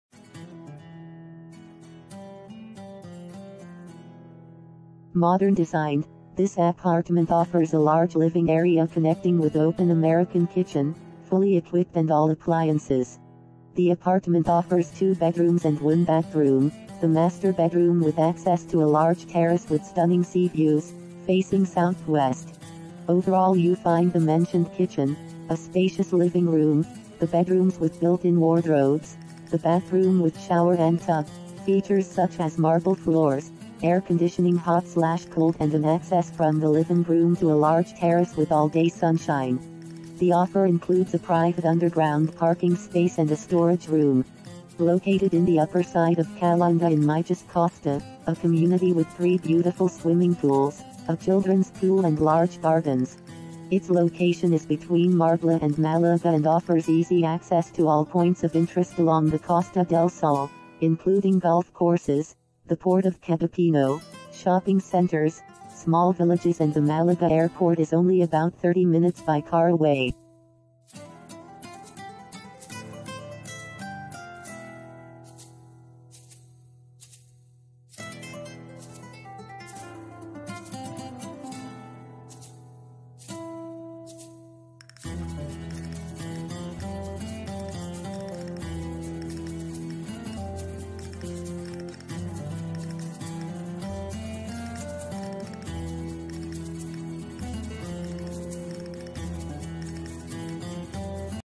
2 Bedrooms Middle Floor Apartment in Calahonda, Costa Del Sol FOR SALE &euro; 245,000
2 Bedrooms Middle Floor Apartment in Calahonda, FOR SALE &euro; 245,000Modern designed, this appartment offers a large living area connecting with a open american kitchen, fully equipped and all appliances. The apartment offers two bedrooms and one bathroom, the master bedroom with access to a large terrace with stunning sea views, facing south west. Overall you find the mentioned kitchen, a spacious living room, the bedrooms with built in wardrobes, the bathroom with shower and tub, features such as marble floors, air conditioning hot/cold and an access from the livingroom to a large terrace with all day sunshine. The offer includes a private underground parking space and a storage room. Located in the upper site of Calahonda in Mijas Costa, a community with three beautiful swimming pools, a children's pool and large gardens. Its location is between Marbella and Málaga and offers easy access to all points of interest along the Costa del Sol, including golf courses, the port of Cabopino, shopping centers, small villages and the Malaga airport is only about 30minutes by car away!Covered Terrace;Lift;Fitted Wardrobes;Private Terrace;Satellite TVADSL/WIFI;Storage Room;Utility Room;Marble Flooring;Double GlazingClose To Golf;Close To Shops;Close To Sea;Urbanisation To view and enquire about this property please